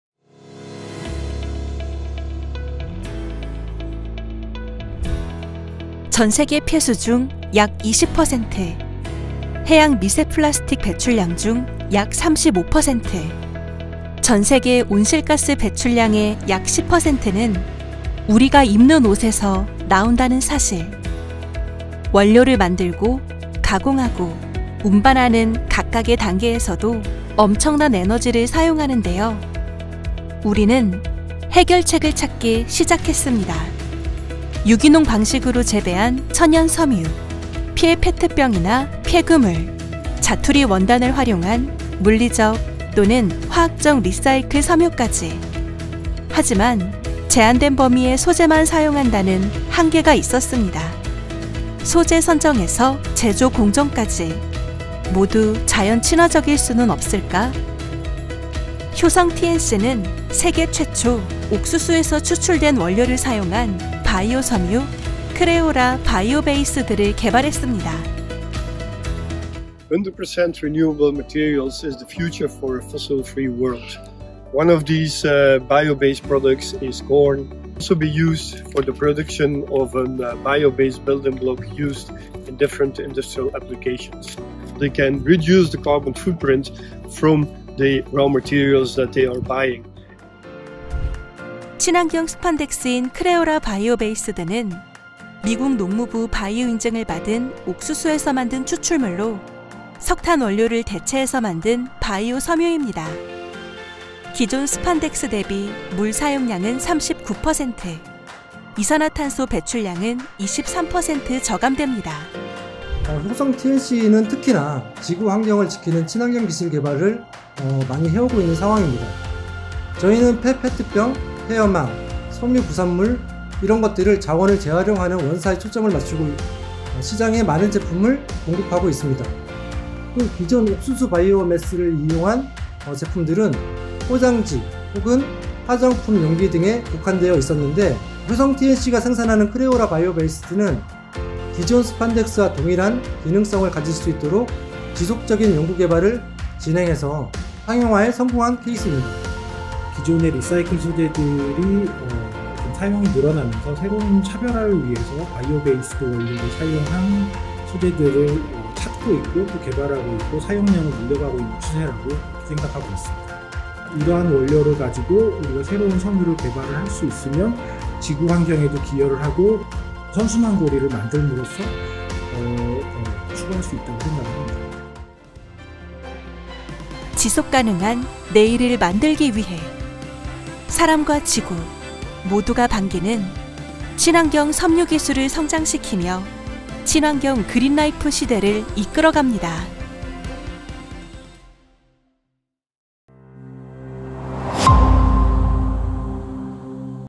나는 옥수수를 입고 다녀요~ | 옥수수로 만든 효성티앤씨 크레오라 바이오베이스드 | 효성TV
효성티앤씨가 세계 최초로 옥수수 추출물을 이용한 크레오라 바이오베이스드를 개발했습니다.
지구 환경에 기여하고 선순환 고리를 만드는 친환경 기술 함께 보실까요?